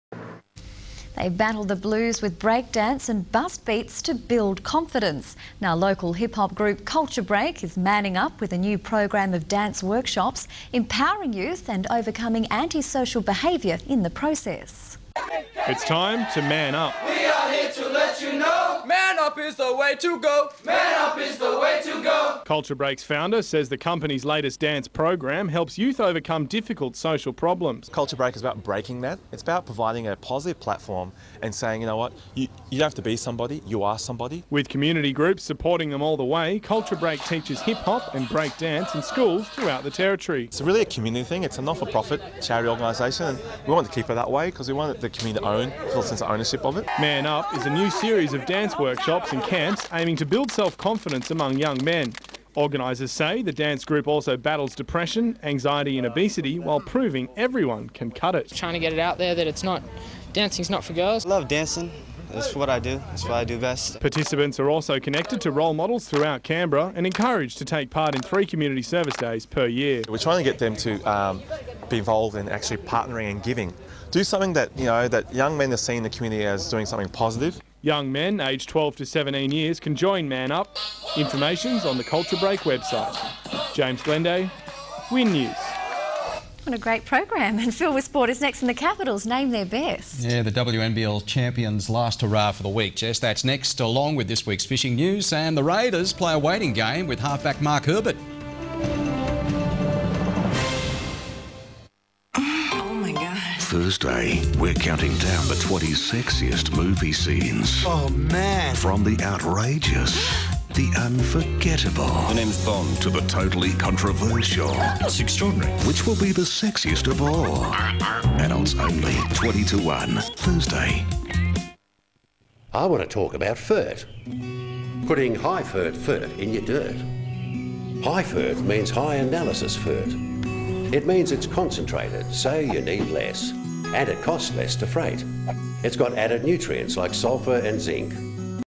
Kulture Break Man up Win News Report
Man- Up program is a risk prevention and re-engagement creative arts mentoring program targeted at school aged males between 12-17yrs. The program uses popular youth culture in Hip-hop and Breakdance to connect young people to themselves, school and their community. It provides an avenue for skill and leadership development, boosts in confidence, self-esteem and improving social awareness and responsibility.